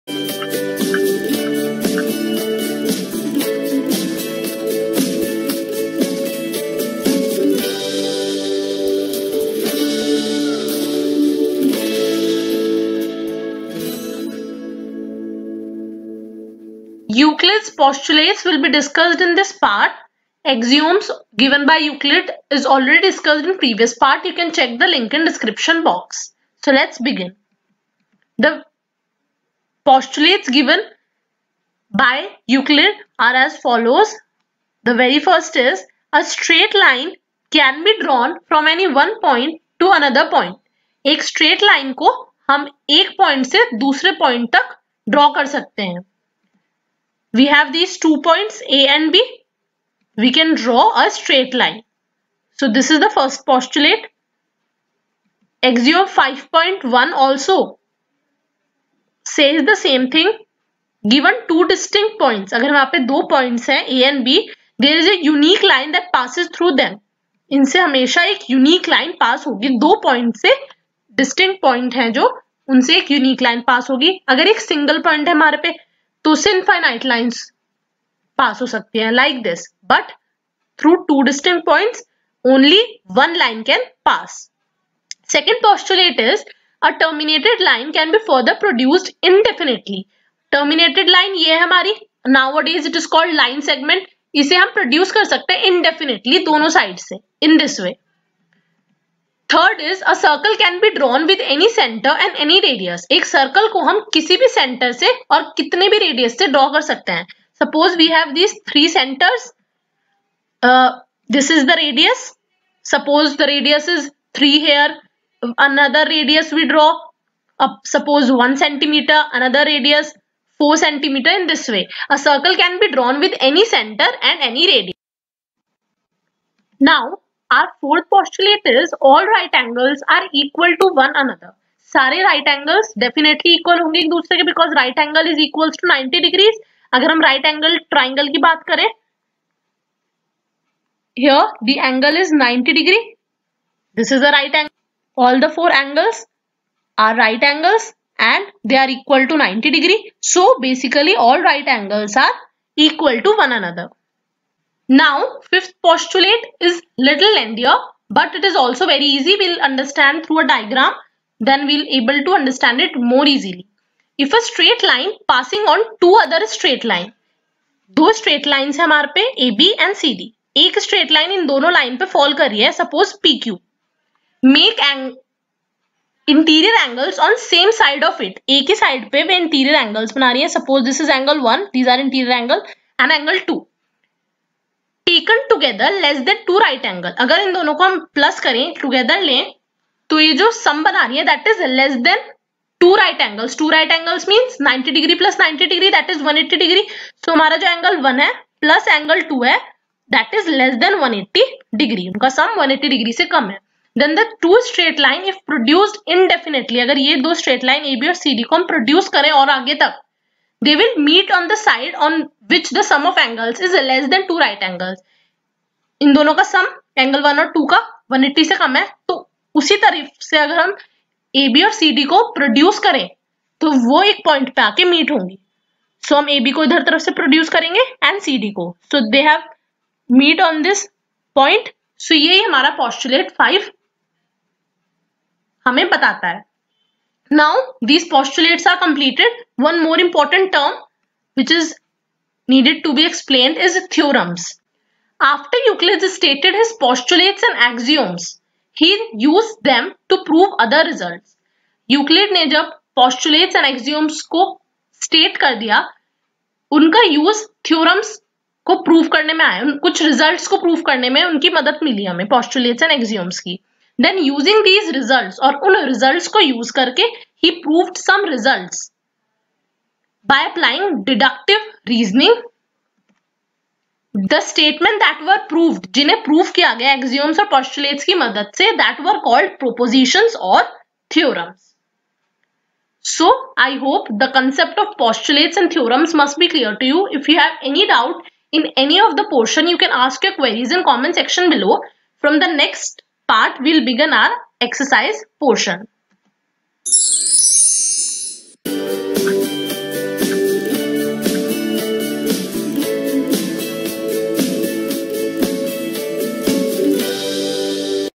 Introduction to Euclid's Geometry|Class9|Maths|Chapter-5|Euclid's Postulates|Part-3|NCERT|CBSE
Aim Of The Channel- Learn By  Focusing on Concepts!!!
Maths Chapter-5 : Introduction to Euclid's Geometry
Here you will get to know:
Detailed explanation of Euclid's Postulates with examples is given.
Meaning of theorems

Also, you can watch entire class 9 chemistry syllabus on our channel through visualization and animations..